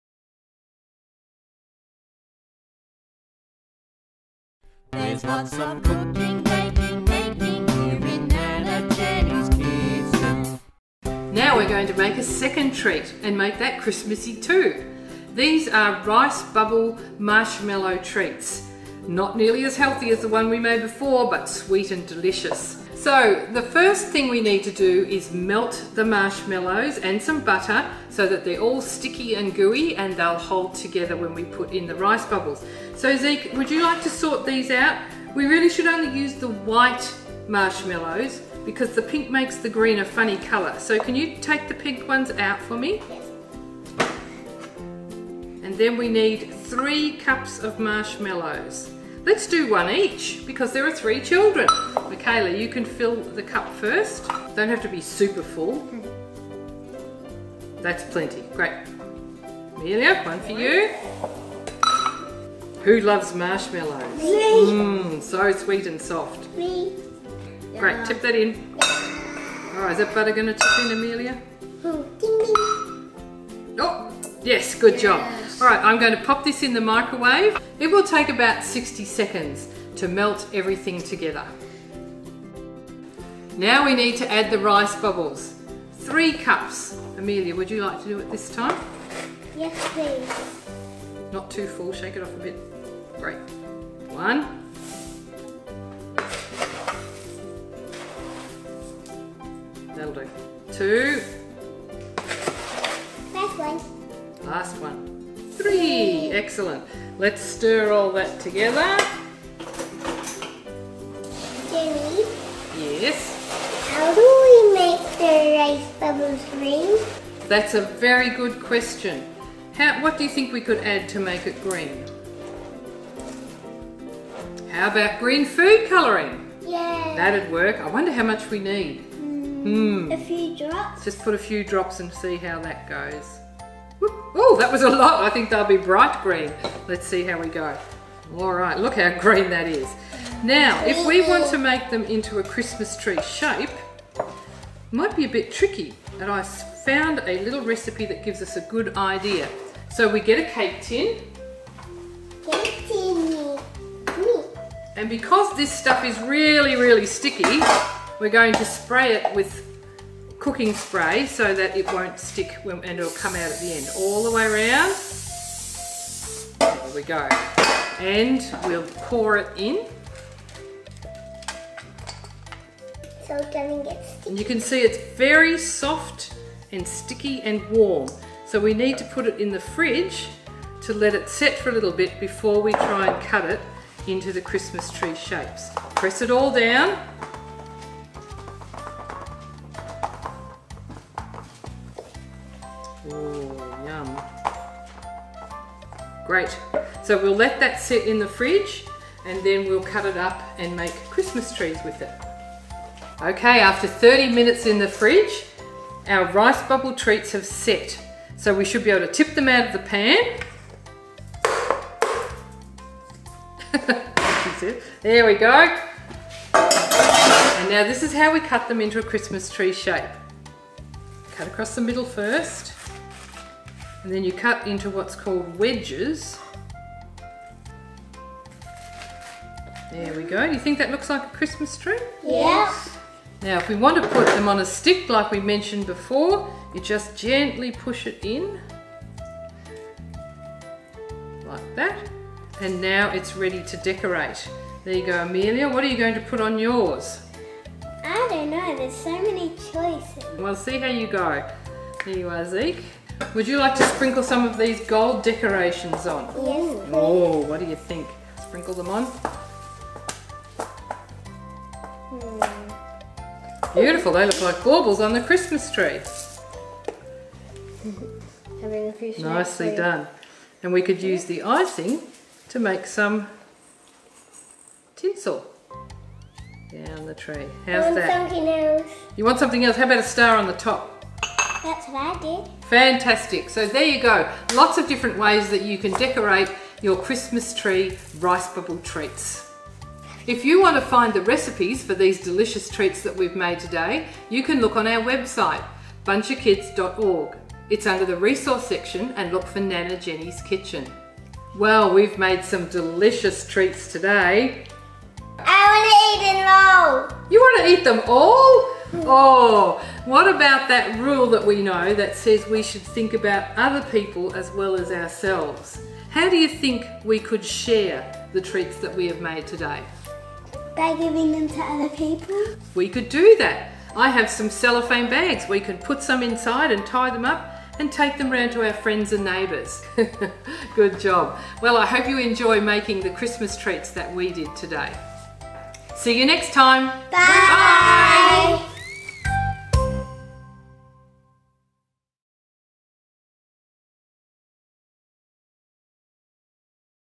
B-OK TV EP 1 Nana Jenny's Rice Bubble Christmas Tree
Learn how to make some amazing Rice Bubble Christmas Tree sweets with Nana Jenny and the kids.